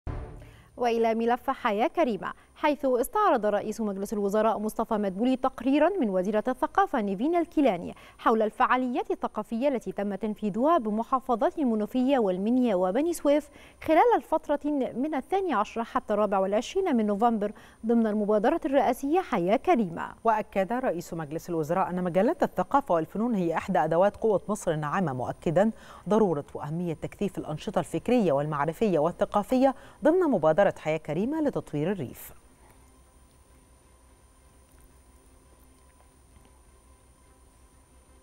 مدبولي يستعرض تقريرا حول الفعاليات الثقافية بالمنوفية والمنيا وبني سويف خلال نوفمبر ضمن حياة كريمة
نشرة الثالثة | مدبولي يستعرض تقريرا حول الفعاليات الثقافية بالمنوفية والمنيا وبني سويف خلال نوفمبر ضمن حياة كريمة